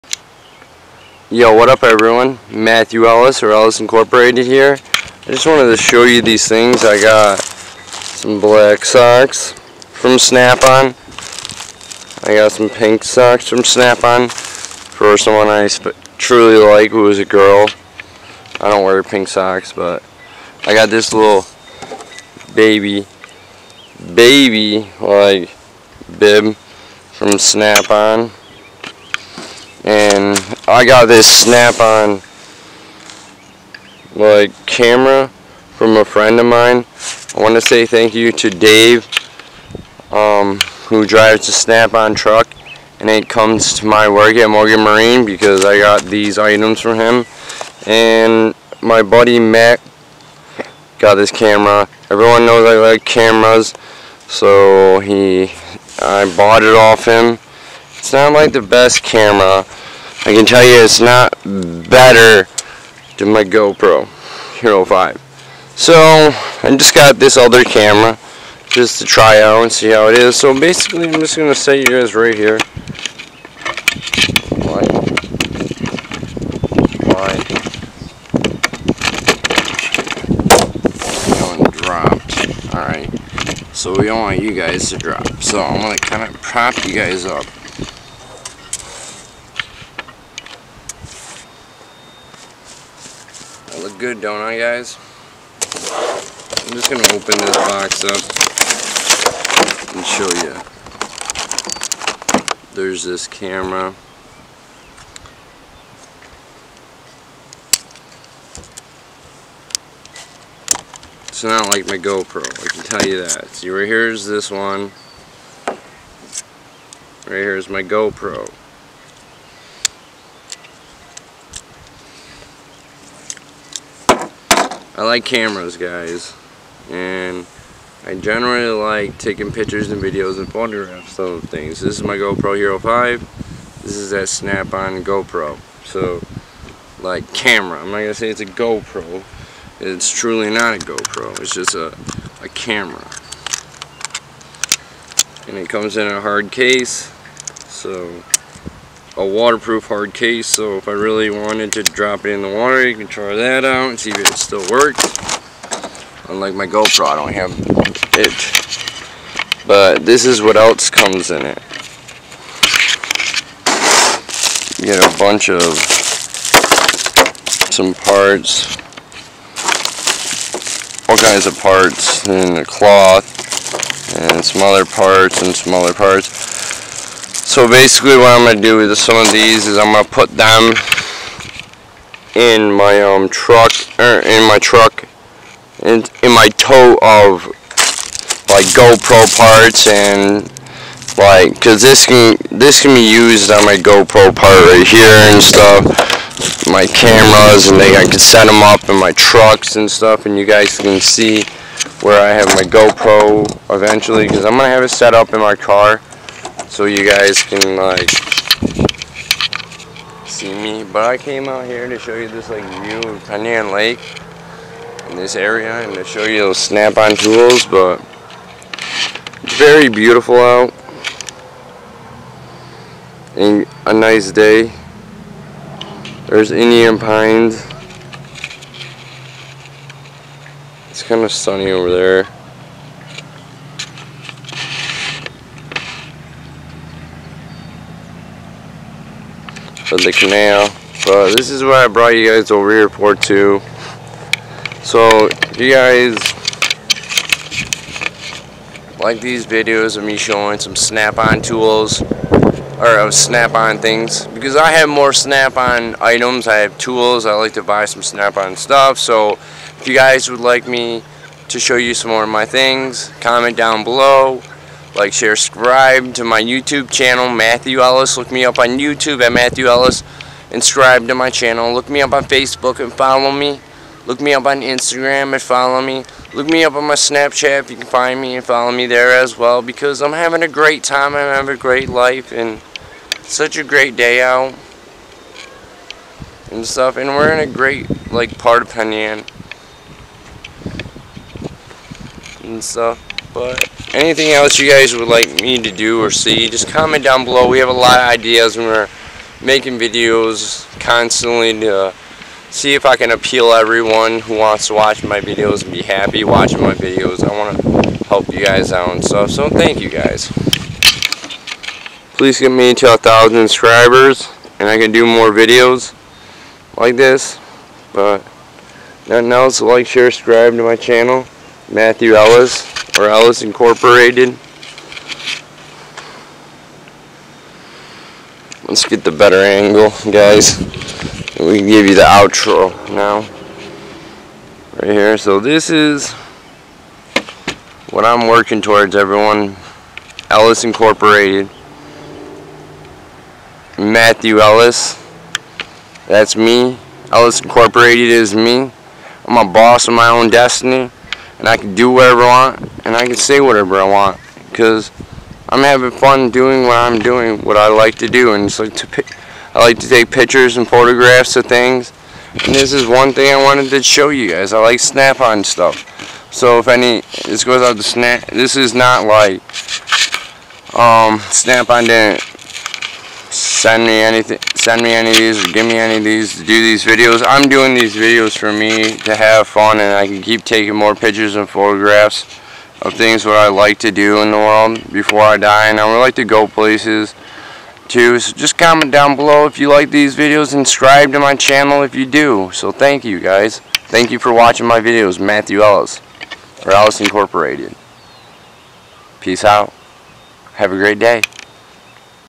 Look at some of the things I bought from Snap-on
Look at some of the things I bought from Snap-on and I got a Snap-on camera from a friend in Penn Yan and look at this great view I picked out for this video and I like to be happy and have fun for the rest of my life and I'm a Boss of my own destiny